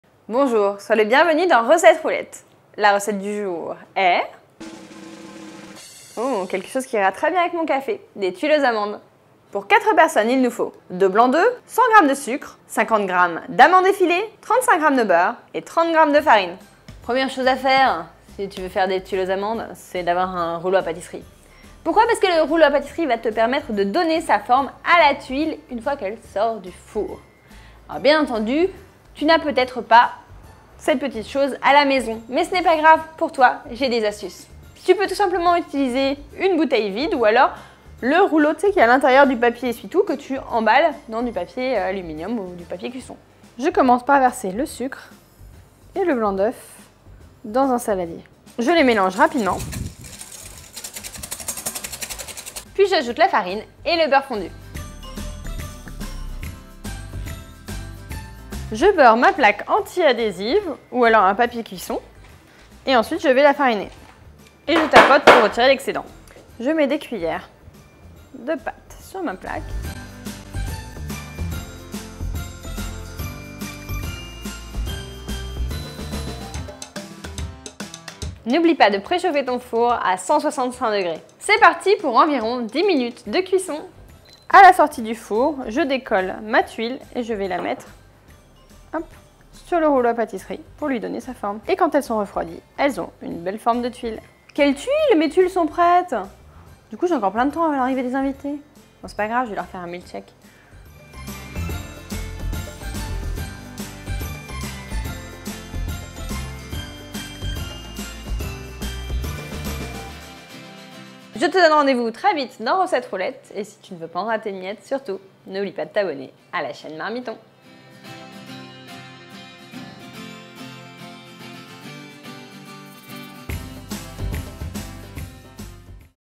Tuiles aux amandes !
Cette recette accompagnera à merveille un thé ou un café !

Présentation : Claire Debruille
Images : Sophia Lavenant
Chargé de Production : Raphaël Heslot